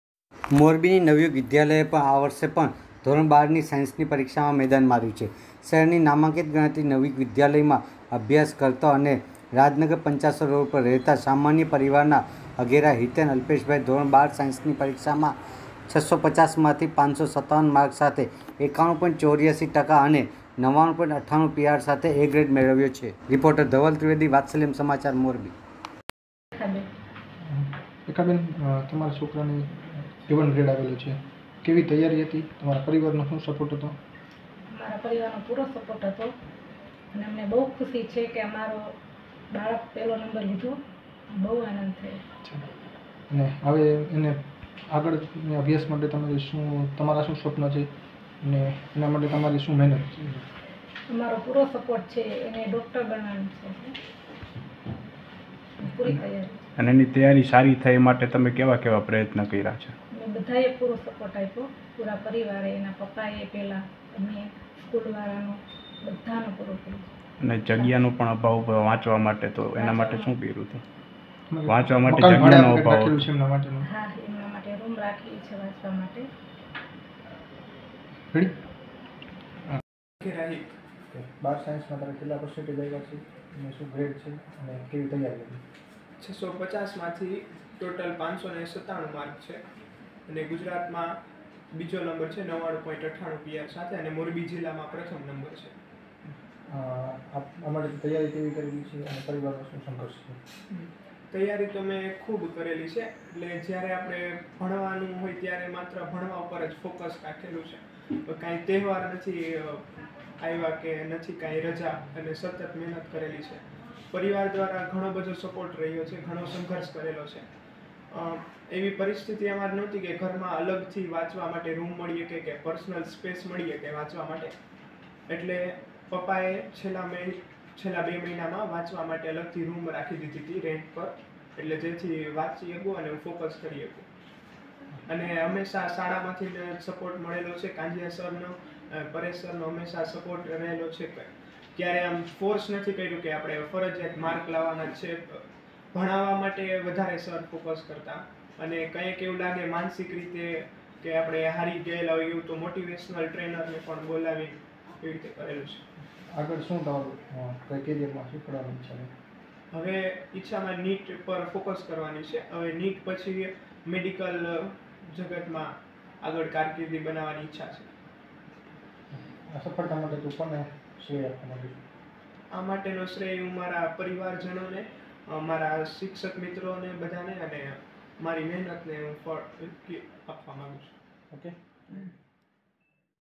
મોરબી ની નવયુગ વિદ્યાલયના વિદ્યાર્થીએ એવન ગ્રેડ મેળવ્યો
મોરબીની નવયુગ વિદ્યાલયએ આ વખતે પણ ધો.12 સાયન્સની પરીક્ષામાં મેદાન માર્યું છે. શહેરની નામાંકિત ગણાતી નવયુગ વિદ્યાલયમાં અભ્યાસ કરતો અને રાજનગર પંચાસર રોડ ઉપર રહેતો સામાન્ય પરિવારના અઘેરા હિત અલ્પેશભાઈએ ધો.12 સાયન્સની પરીક્ષામાં 650માંથી 597 માર્ક્સ સાથે 91.84 ટકા અને 99.98 પીઆર સાથે એવન ગ્રેડ મેળવ્યો છે. તેને સૌથી વધુ બાયોલોજીમાં 96 માર્ક્સ આવ્યા છે. જ્યારે ગુજકેટમાં 115 માર્ક્સ આવ્યા છે. તેના પિતા સીરામીક કારખાનામાં નોકરી કરે છે. મોટાભાગે હિત ઘરે જ પરીક્ષાની તૈયારી કરતો અને દરરોજ આઠ કલાકથી વધુ અભ્યાસ કરતો આ ઉપરાંત યોગોમાં જિલ્લામાં ત્રણ વખત નંબર મેળવ્યો છે. તેમજ એનસીસીમાં બે મેડલ મેળવ્યા છે. એટલે અભ્યાસની સાથે બીજી પ્રવૃતિઓમાં પણ તે ઝળકયો છે. તે કહે છે કે, યોગા કરવાથી યાદશક્તિ વધુ સારી રહે છે. તે સામાન્ય પરિવાર હોય ઘર પણ નાનું હોય તેમજ સંયુક્ત પરિવાર હોવાથી વાંચવા માટે અલગ રૂમ ન હોવા છતાં તેણે સખત મહેનત કરીને ઉજ્જવળ પરિણામ મેળવ્યું છે. હવે તેને નિટની એક્ઝામ આપી એમબીબીએસ થવાની ઈચ્છા છે.